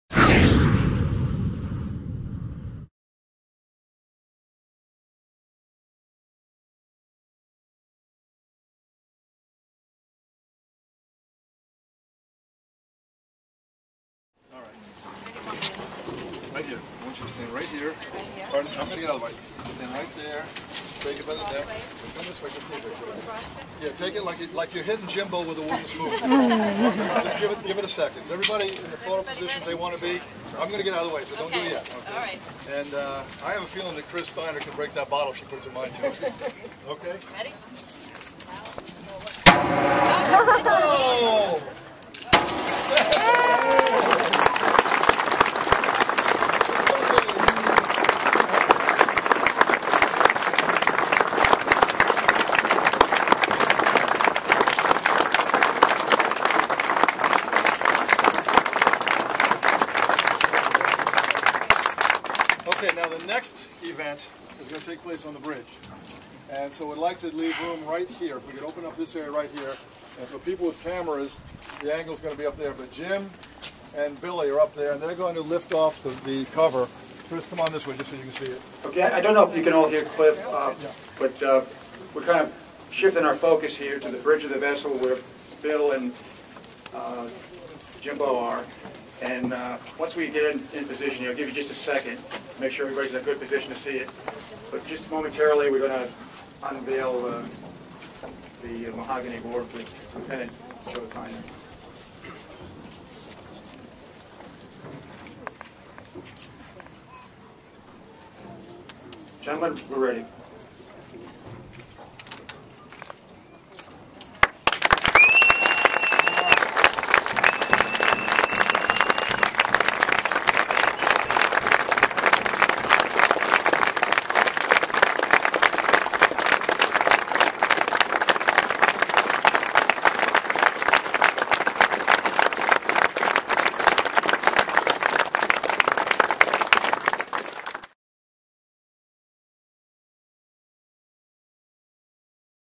Christening of Lt. Joe Theinert Ferry Boat
On July 3, 2010, a South Ferry boat was renamed in honor of Lt. Joe Theinert.  Chrystyna Kestler,  Joe's mother, christened the vessel.